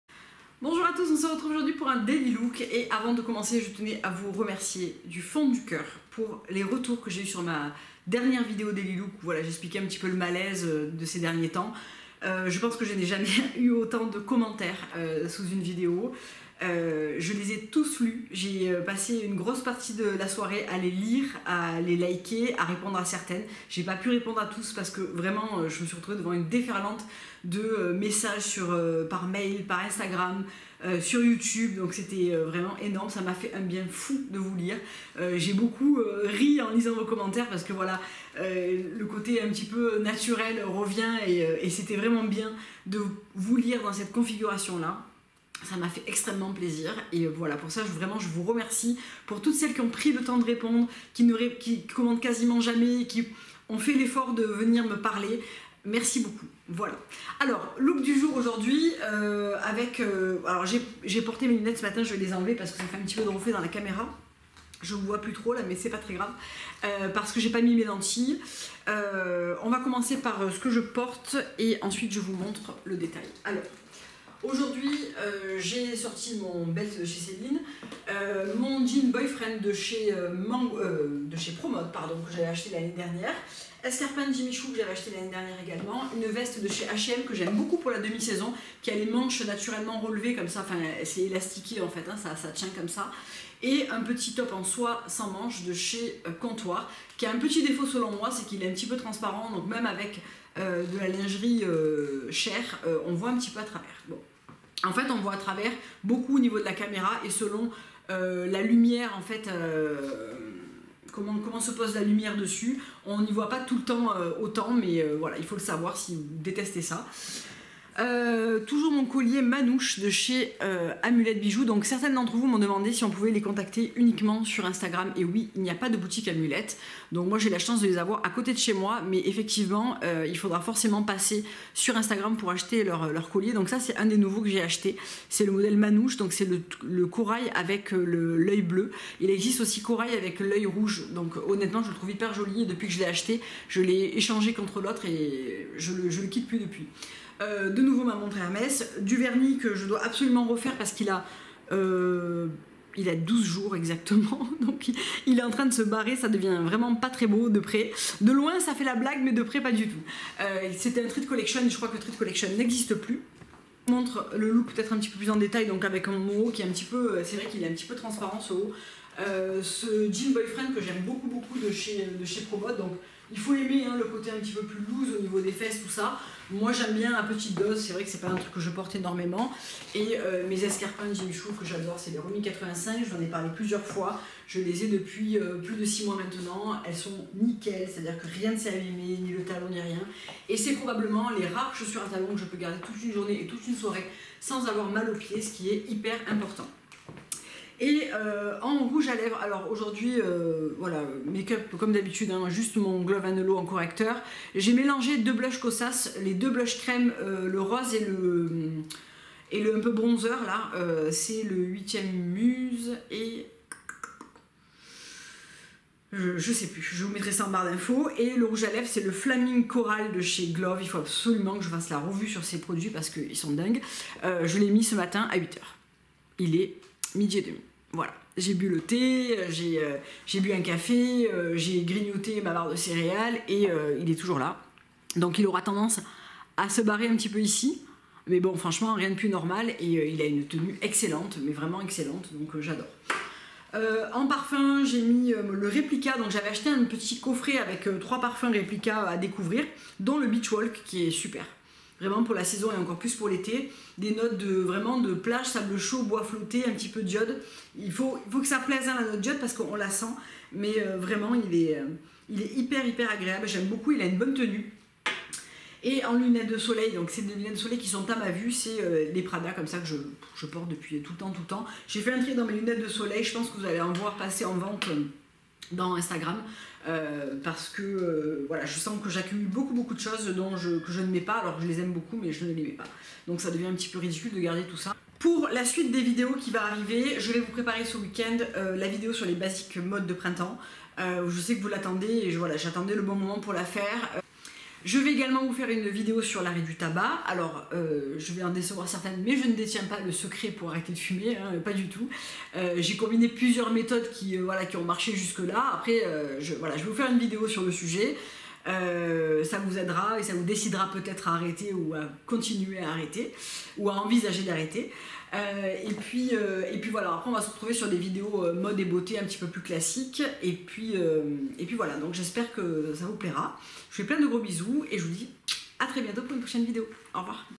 Daily Look ❤️
Ce que je porte :

Veste H&M
Blouse Comptoir des Cotonniers (old) taille 36
Jean Promod  (34)
Collier Amulette Bijoux
Modele Manouche
Vernis A special Something Treat Collection

Jimmy Choo  Romy 85 (37 1/2 )

Sac Belt Celine Light Taupe medium

Parfum Beach Walk Replica (Maison Martin  Margiela)

Makeup :

Anticernes Sappho et Clove (teinte medium et 4 )
Blush Kosas melange de tropic equinox et 8th Muse
Palette Noisette Antonym et mascara Une Beauty
Lipstick Clove and Hallow Flaming Coral

Lunettes de Soleil Prada (old)

Ceinture Comptoir des Cotonniers (old)